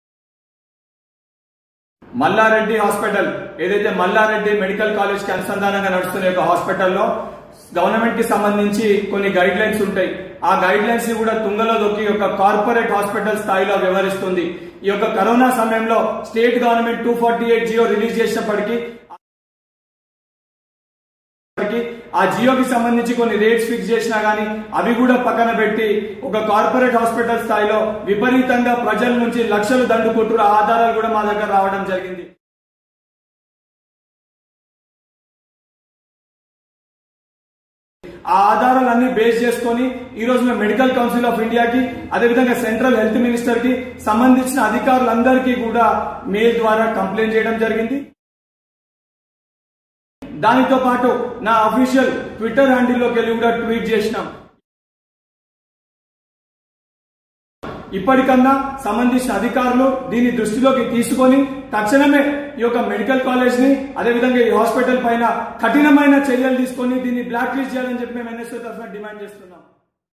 Complaint lodged against Malla Reddy Hospital to Medical Council | Venkat Balmoor | Rangareddy NSUI
NSUI (National Students Union of India)

India's Largest Secular and Democratic Student Organization.
This is the Official YouTube Handle of NSUI Rangareddy District..

NSUI RANGAREDDY DISTRICT SOCIAL MEDIA PLATFORMS